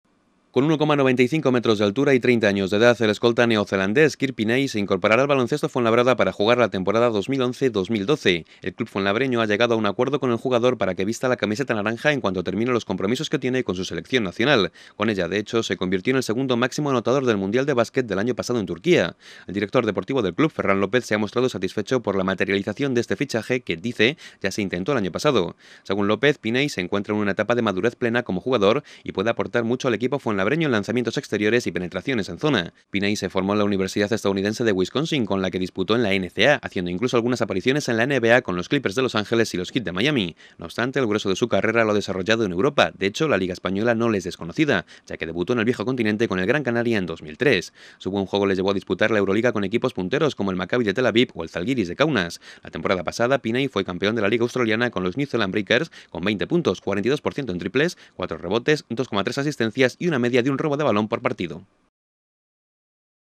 El Fuenlabrada ficha al segundo máximo anotador del Mundial de Turquía, Kirk Penney
El Club Baloncesto Fuenlabrada ha llegado a un acuerdo con el jugador neozelandés Kirk Penney, de 30 años, para que juegue aquí la próxima temporada. Penney, que se incorporará al equipo cuando finalice sus compromisos con su selección nacional fue el segundo máximo anotador el Mundial de Turquía el año pasado, logrando una media de 24'7 puntos por partido.